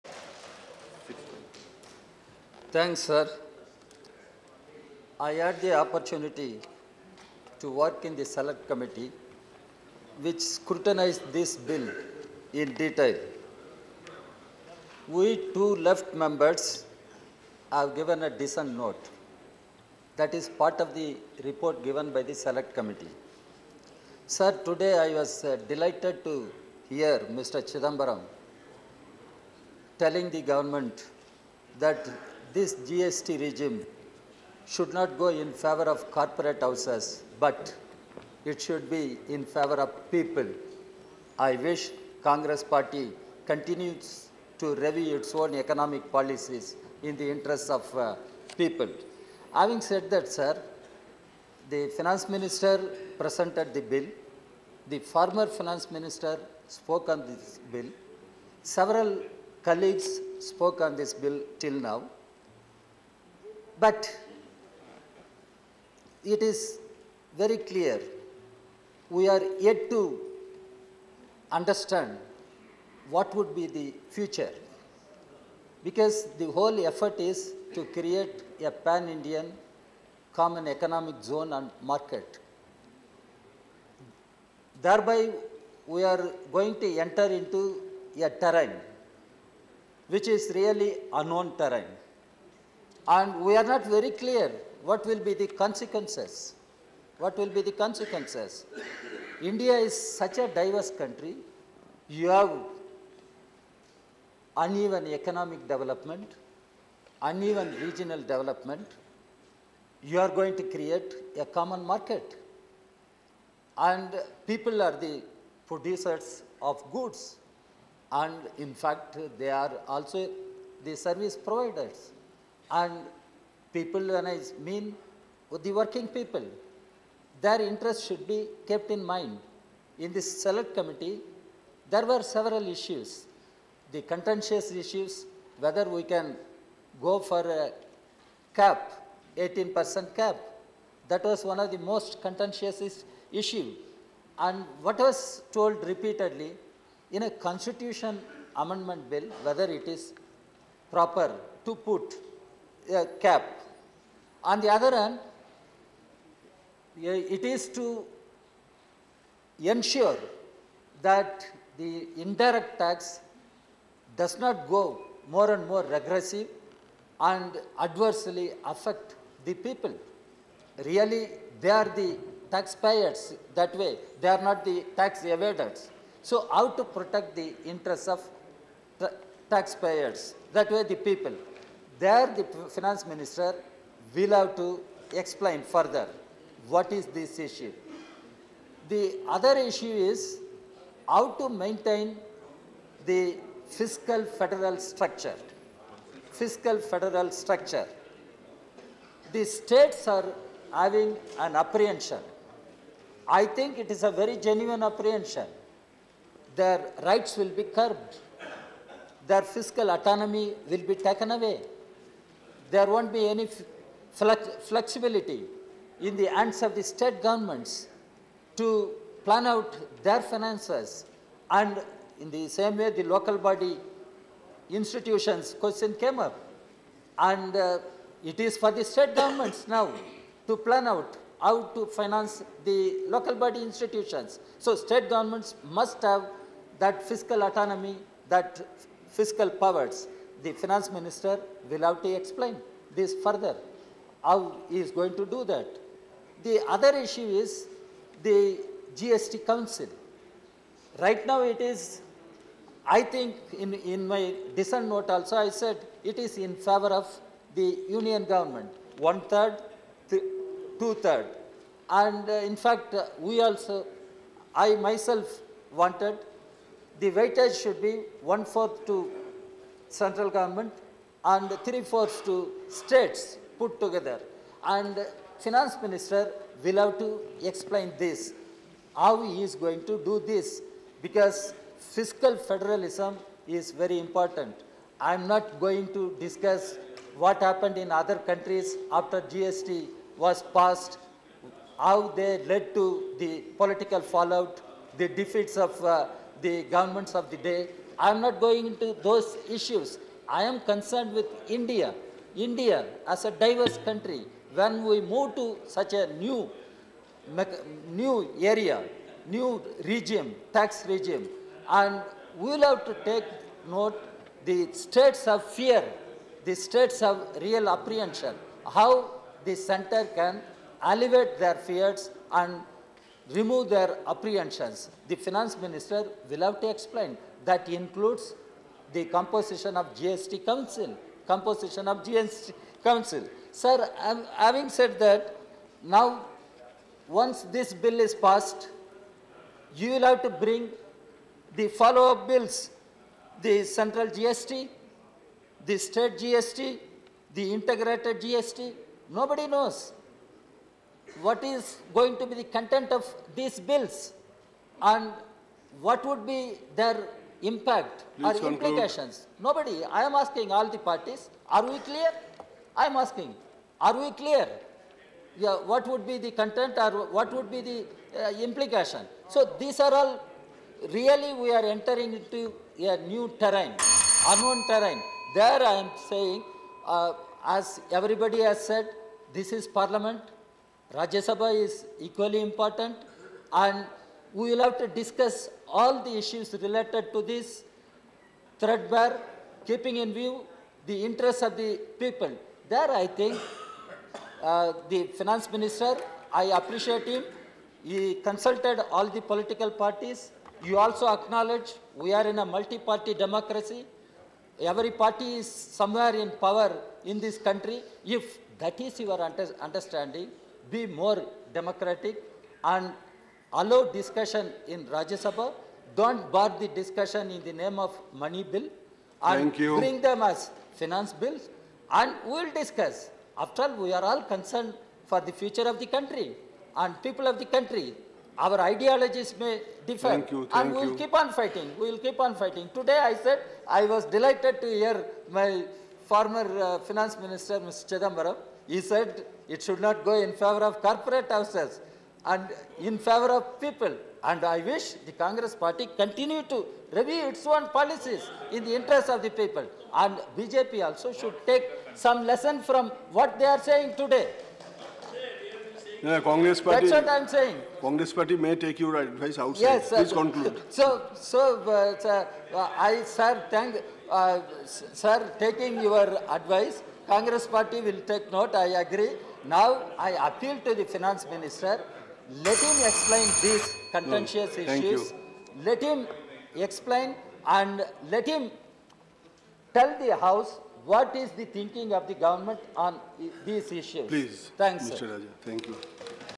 Sh. D Raja’s comments on The Constitution (122nd Amnd.) [GST] Bill, 2014
Discussion on The Constitution (One Hundred and Twenty-second Amendment) Bill, 2014.
Date: Aug 3, 2016 | Rajya Sabha Monsoon Session - 240